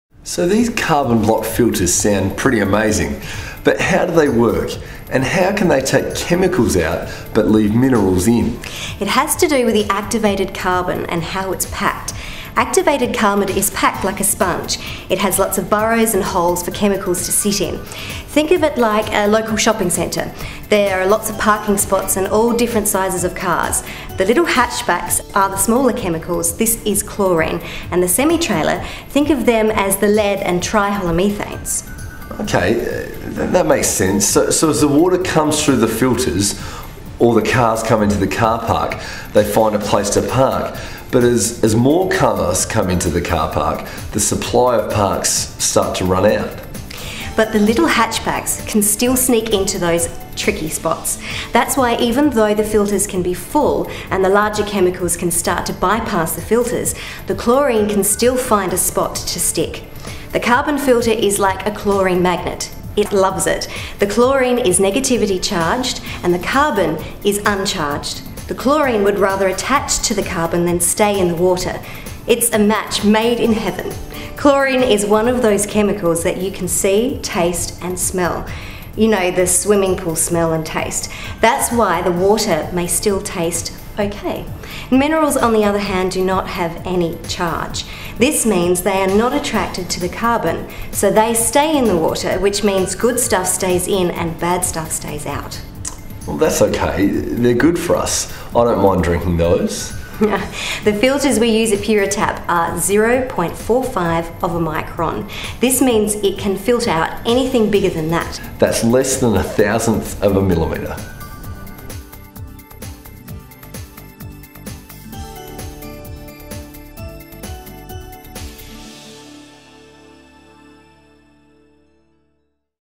How does a carbon filter work?
You have probably heard about them and all the chemicals and contaminants they can remove from your water, but how does a carbon filter work?